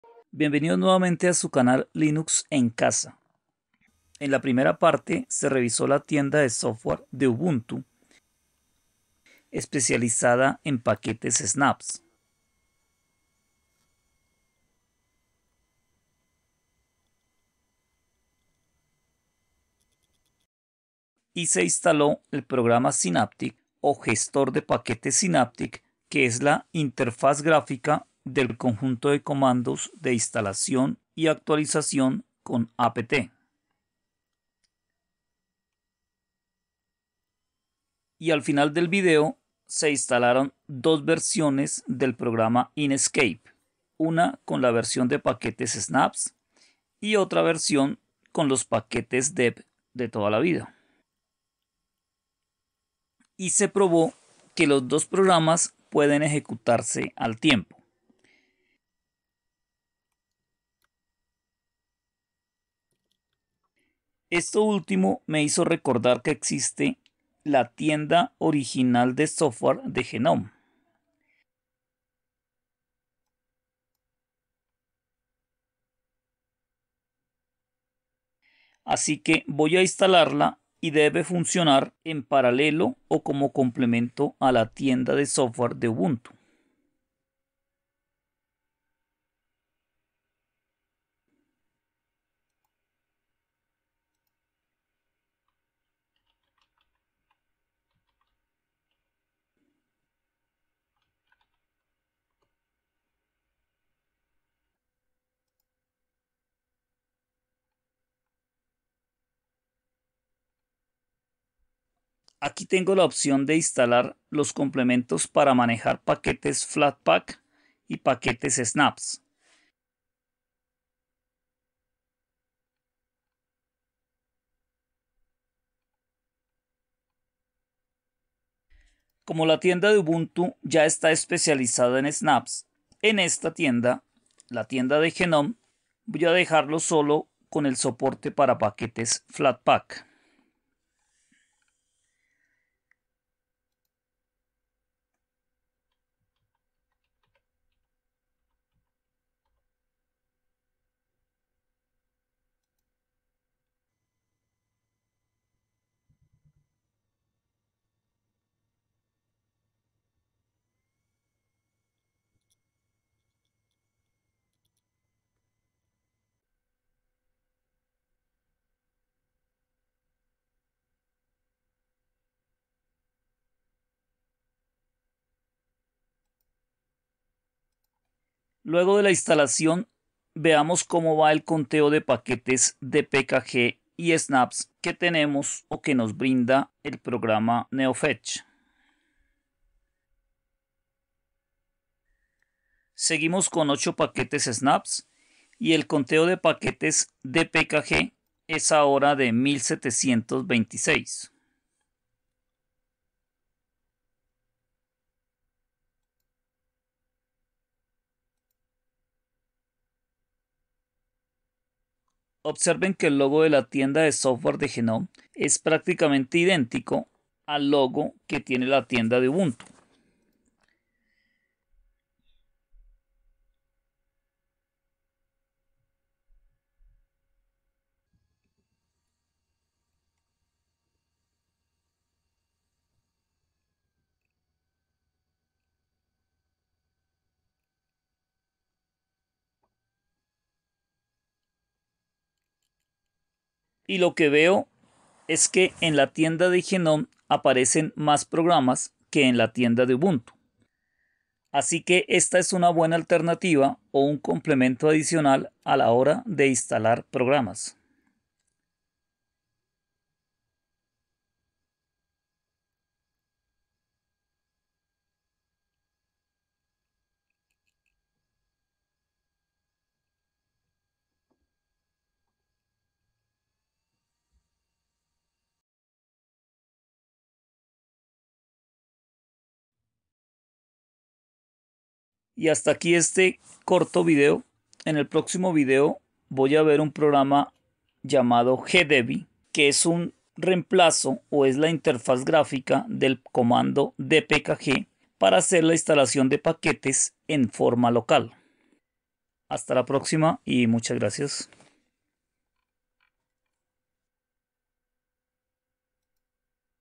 PU-02 - INSTALAR PROGRAMAS en UBUNTU LINUX – gnome-software - Tienda de Software de GNOME [V065]
En esta segunda parte sobre la Instalación de Programas en UBUNTU LINUX se instala La Tienda de Software de GNOME o gnome-software.

## DETALLES TECNICOS:
Prueba realzada en Oracle VM VirtualBox.
SO. Ubuntu 20.04 LTS "Focal Fossa" - 64 bits.